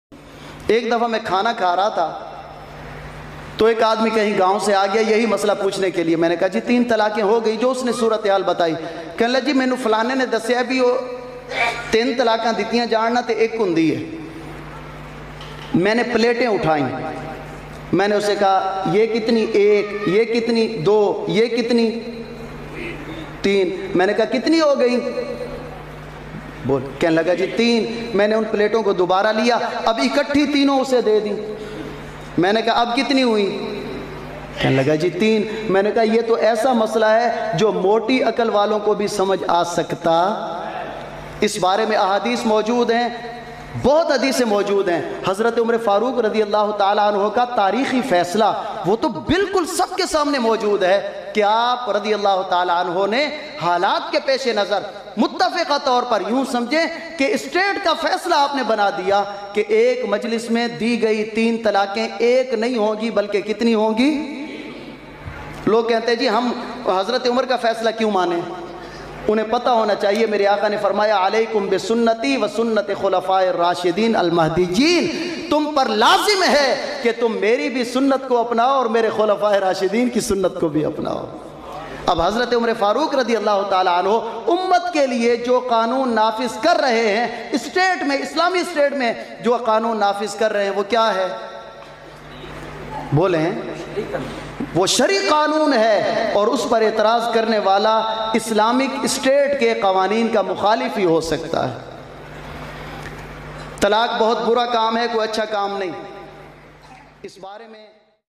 Talaak aur hazart Umar e farooq RA ka tarikhi fasla.kya aik hi bar min fi gai talkin ho gai faizan.
Talaak aur hazart Umar e farooq RA ka tarikhi fasla.kya aik hi bar min fi gai talkin ho gain.